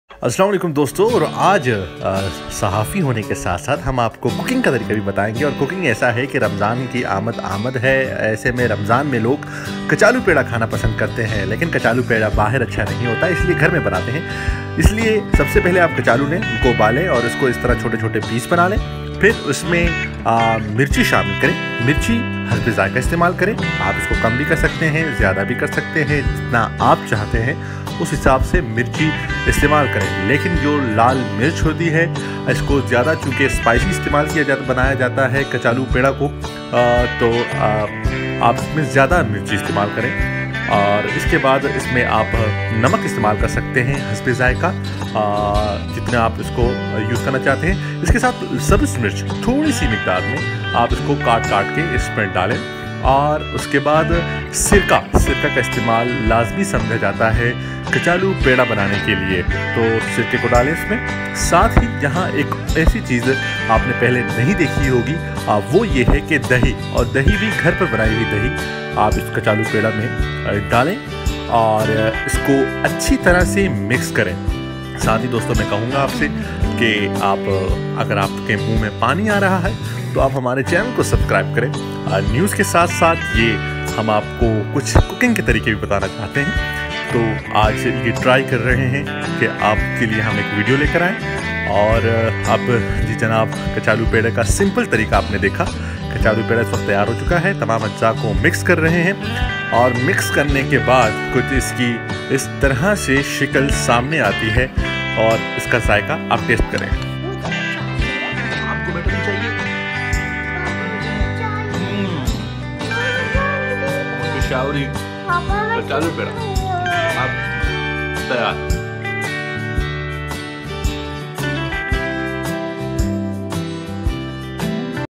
Peshawari kachalu pera Chaat
Its one of peshawar famous recipe which is well known to everyone in the city. So before ramzan everyone should learn how to made this chaat and avoid to buy from outside which is unhygienic.

600 years old mandir for hindu community in Peshawar